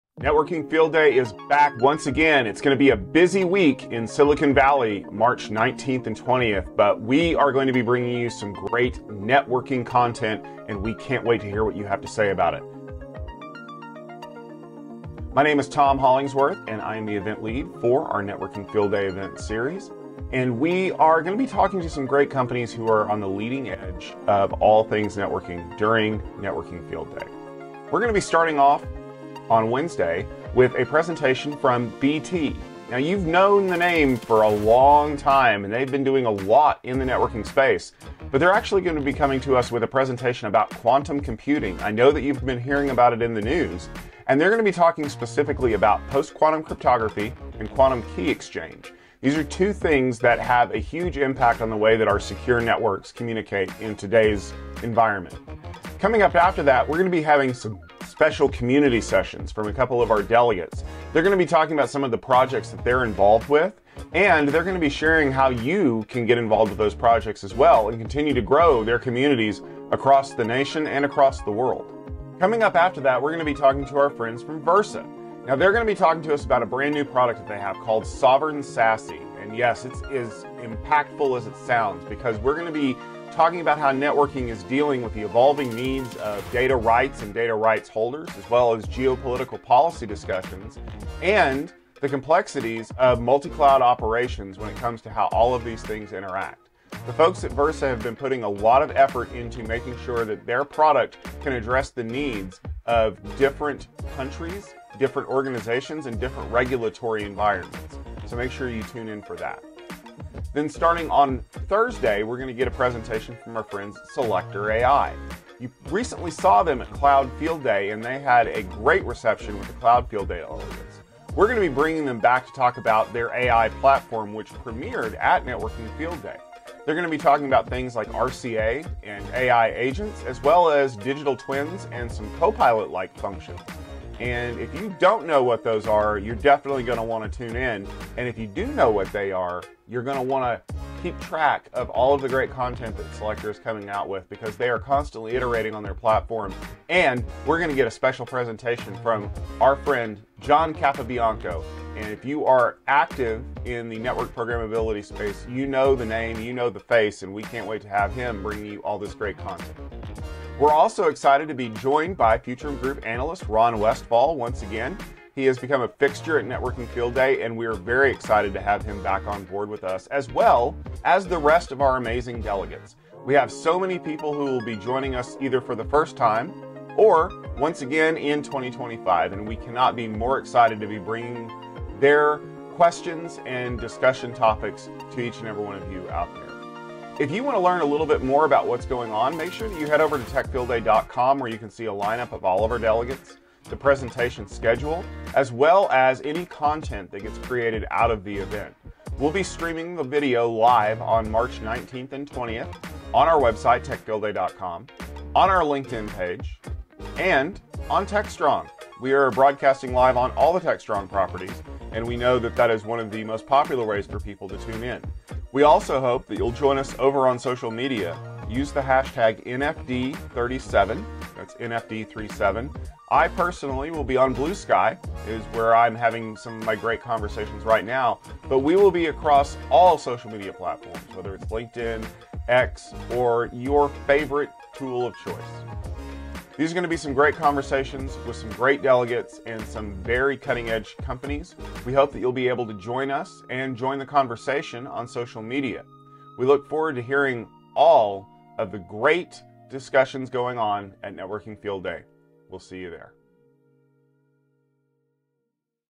The Return of Networking Field Day
Networking Field Day is back! One of most popular events is bringing you the latest edition with a great lineup of companies to discuss the latest in technology when it comes to moving bits back and forth. The topics will be diverse and we hope that you're ready to strap in for the ride.